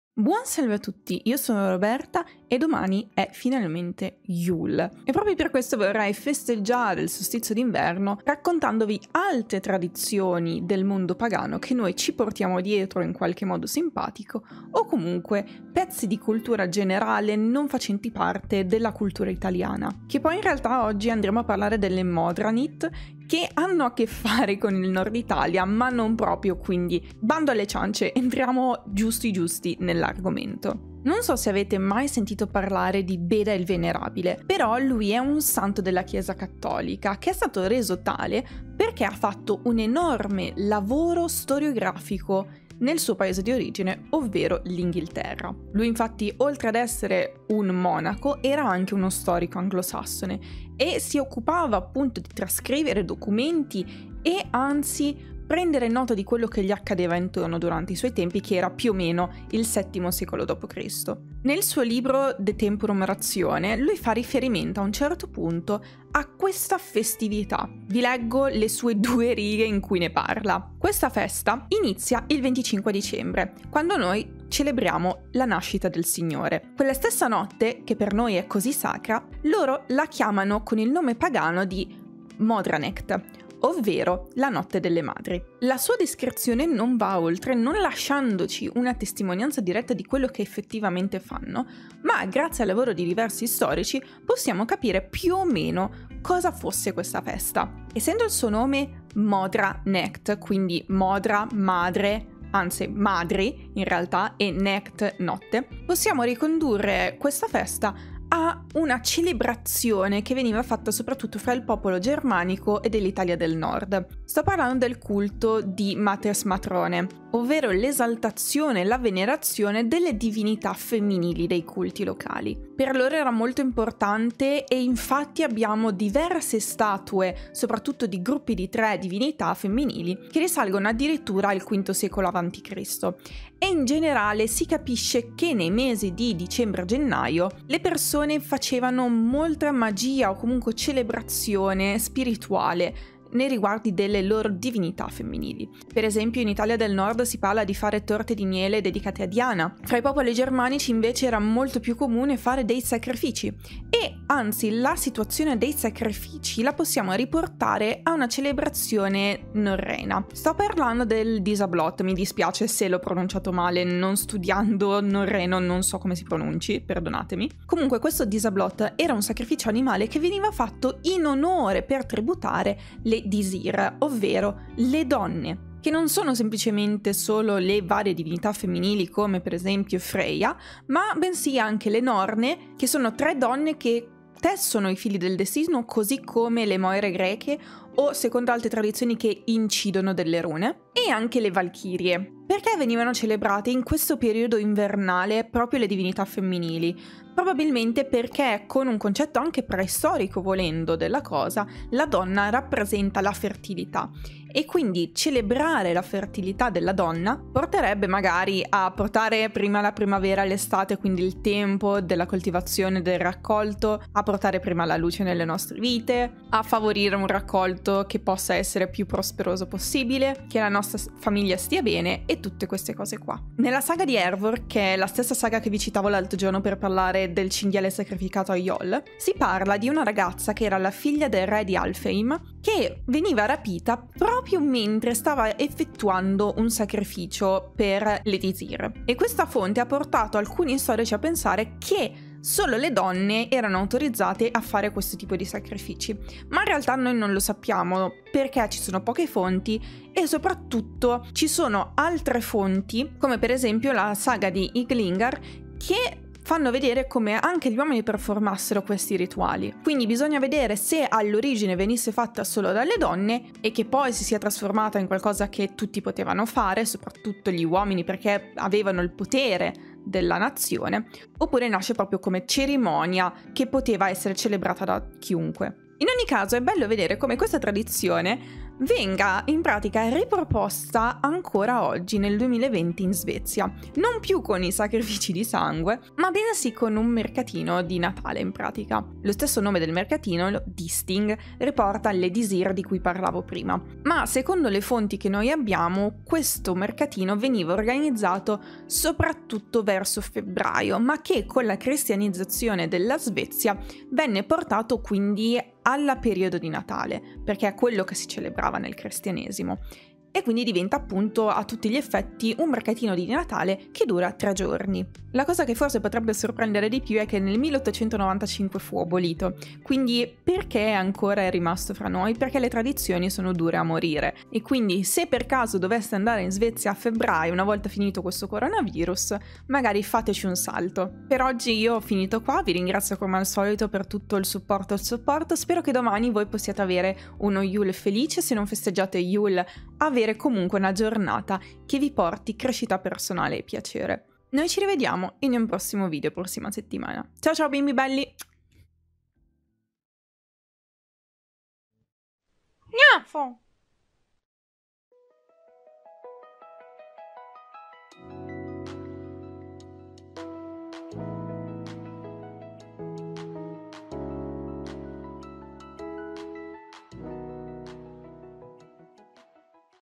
Celebrare le donne a Natale: la notte delle Madri
Oggi parliamo di alcune tradizioni antiche che si festeggiavano intorno al solstizio di inverno: inizieremo con la Modranecht (Notte delle madri) per poi collegarla al culto delle Matres Matronae latine e al Disablot norreno.

Fonti
-Beda, De temporum ratione
-Hervor saga
-Saga degli Ynglingar
-Víga-Glúms saga
-Simek, Dictionary of Northern Mythology

Libri magici consigliati
Wishlist (se comprate da qui, mi arriverà l'oggeto e lo recensirò sul canle)

Gnaffo